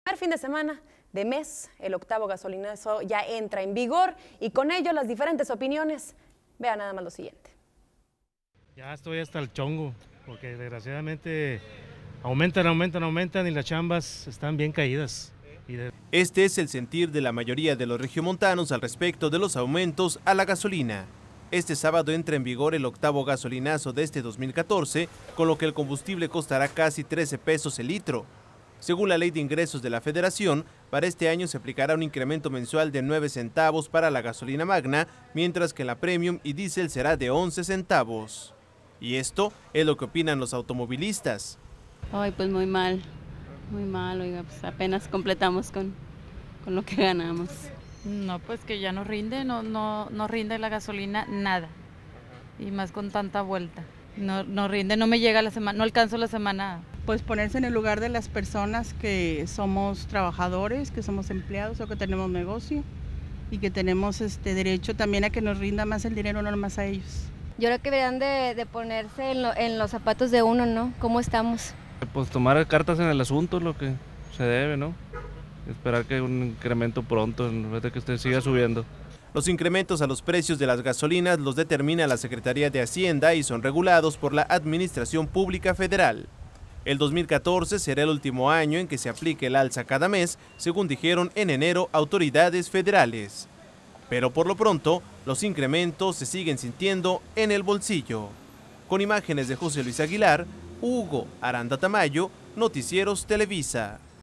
Las Noticias - Opinan regios sobre octavo gasolinazo
Este es el sentir de la mayoría de los regiomontanos al respecto de los aumentos a la gasolina